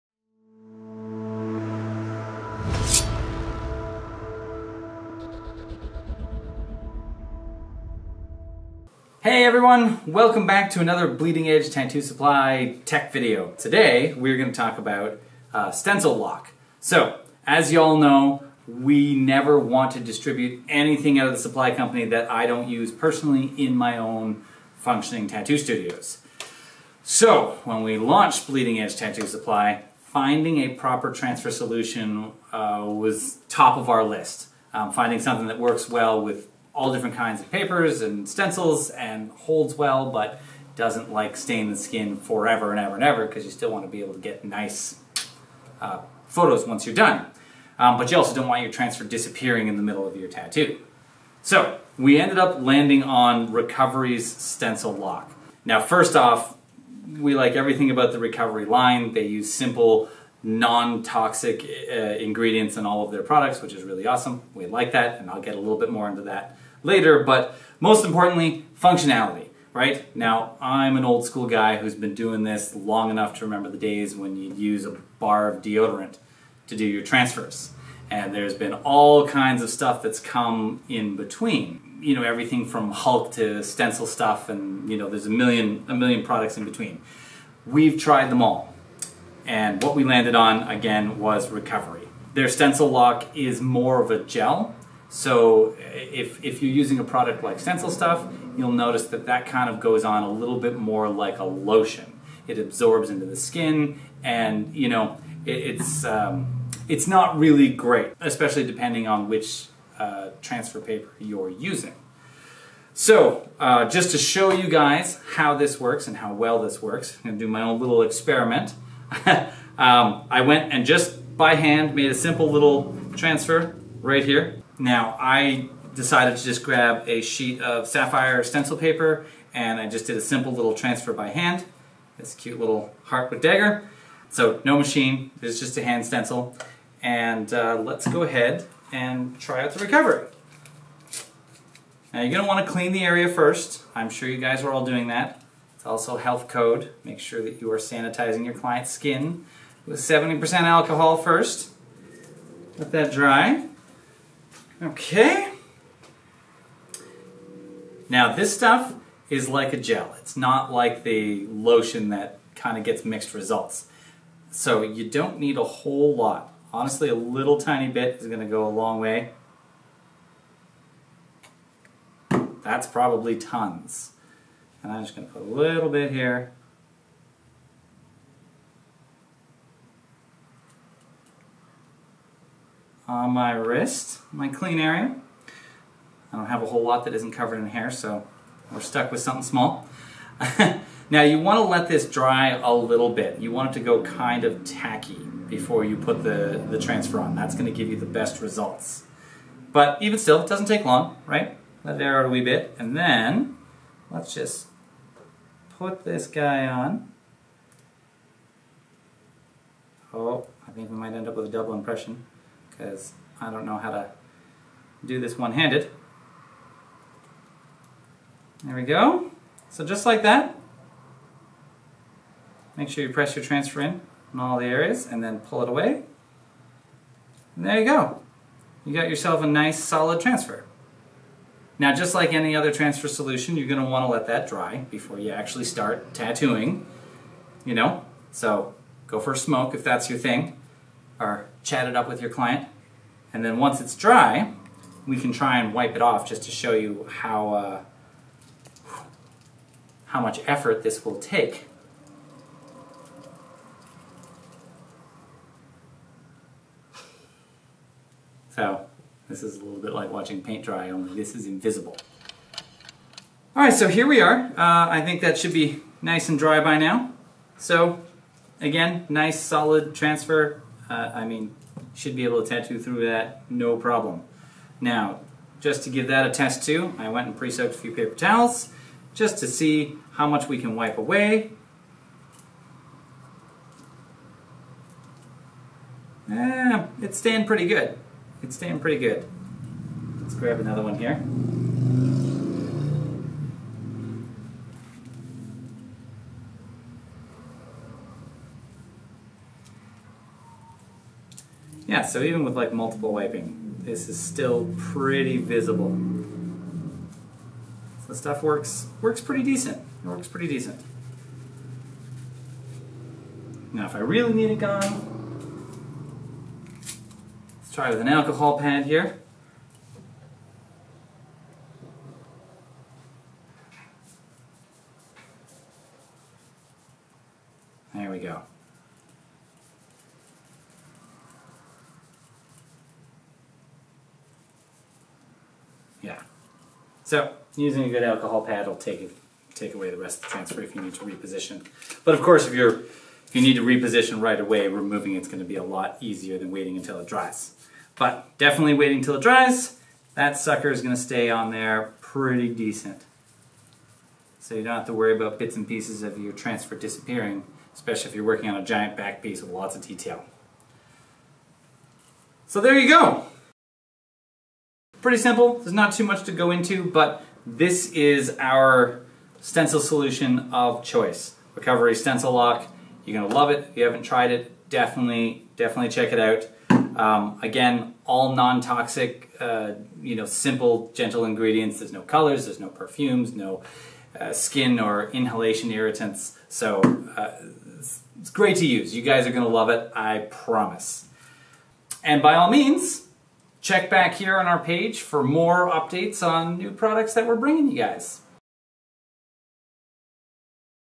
Recovery Stencil Lock
Looking for a great stencil transfer solution?  Well look no further that Recovery's Stencil Lock.  Stencil Lock's vegan friendly, advanced stencil application cream is formulated to enhance tattoo stencil transfers. Manufactured right here in Canada, Stencil Lock creates clean, precise lines with improved longevity, allowing the transfer to last throughout the entire tattoo procedure without smudging or sliding. Designed to work effectively and safely with a variety of rinse solutions, Stencil Lock produces a detailed transfer with only a few drops.  Stencil Lock is made with non-toxic ingredients and does not contain fragrances, colorants, parabens, or other harmful skin and respiratory irritants, making it an essential part of a healthy and skin-friendly tattooing process.